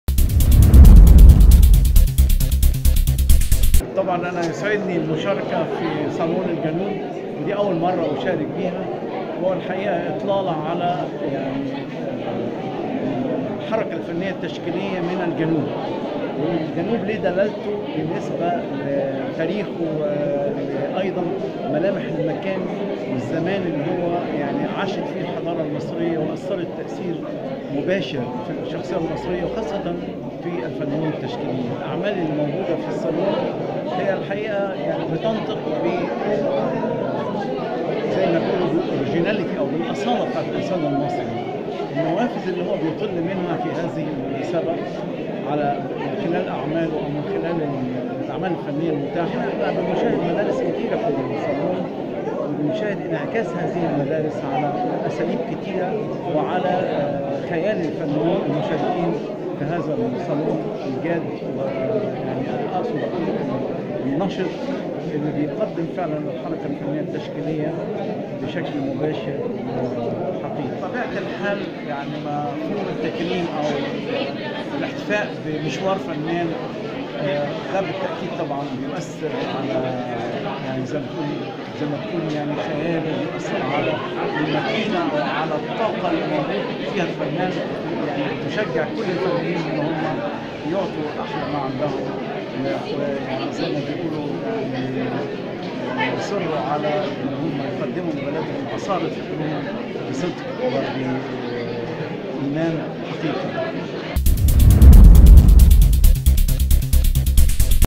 ولاد البلد | عبد السلام عيد: الأعمال المشاركة بصالون الفنون تعبر عن أصالة الإنسان المصري
كرّم صالون الجنوب الدولي بالأقصر، الدكتور عبد السلام عيد، أستاذ التصوير بجامعة الإسكندرية، وضيف شرف الصالون في دورته السادسة، كأحد أهم فناني مصر والعالم العربي.
وقال عيد إن أعمال الفنانين هذه الدورة تنطق بإصالة المصري القديم الذي علم العالم كافة الفنون.